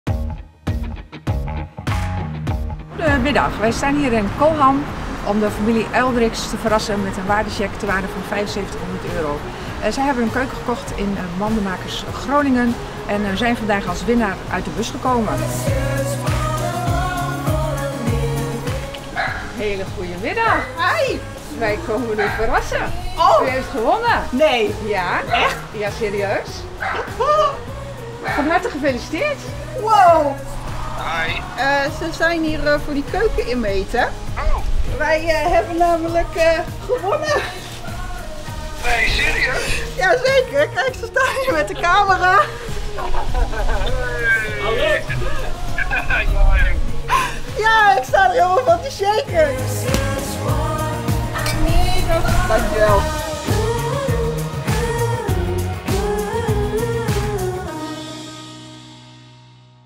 Mandemakers Marathon | Dagwinnaar 5
Tijdens de Mandemakers Marathon maak je élke dag kans op een gratis keuken. De 5e dagwinnaar is de familie Uildriks uit Kolham. Gefeliciteerd met jullie nieuwe keuken!